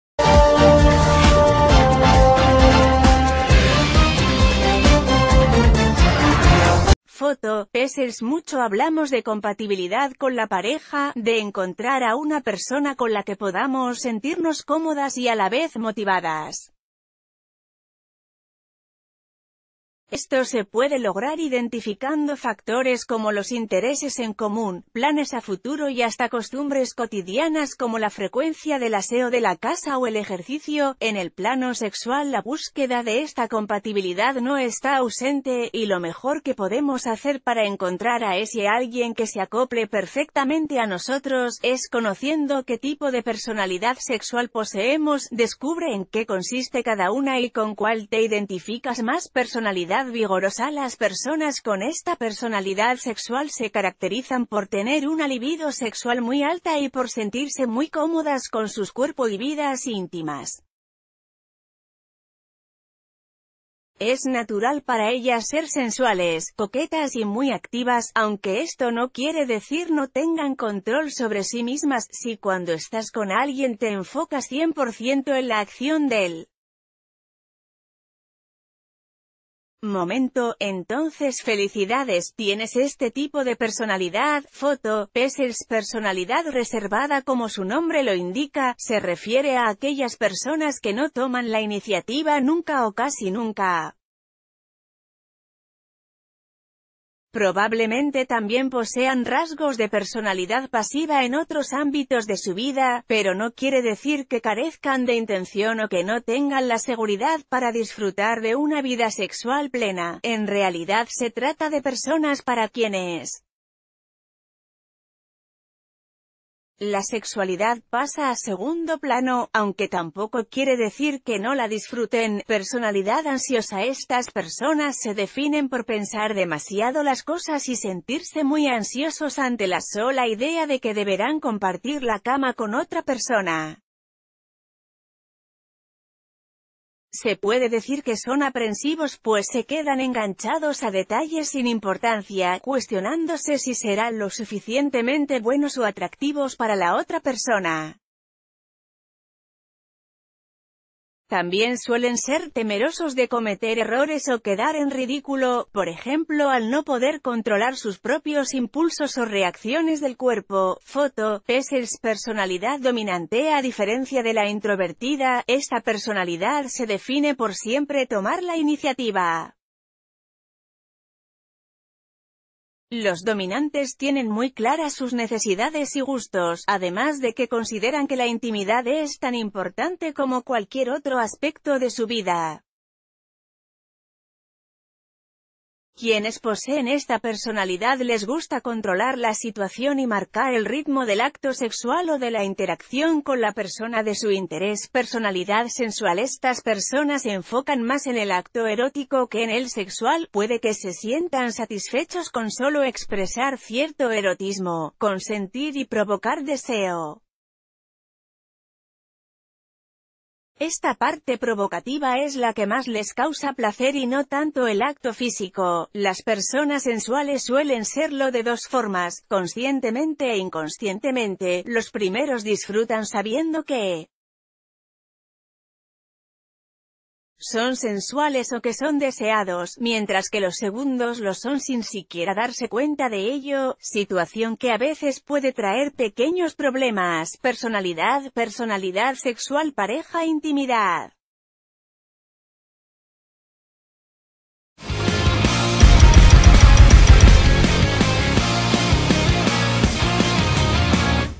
Qué personalidad sexual posees según la psicología
Foto: Pexels Mucho hablamos de compatibilidad con la pareja, de encontrar a una persona con la que podamos sentirnos cómodas y a la vez motivadas. Esto se puede lograr identificando factores como los intereses en común, planes a futuro y hasta costumbres cotidianas como la frecuencia del aseo de la ...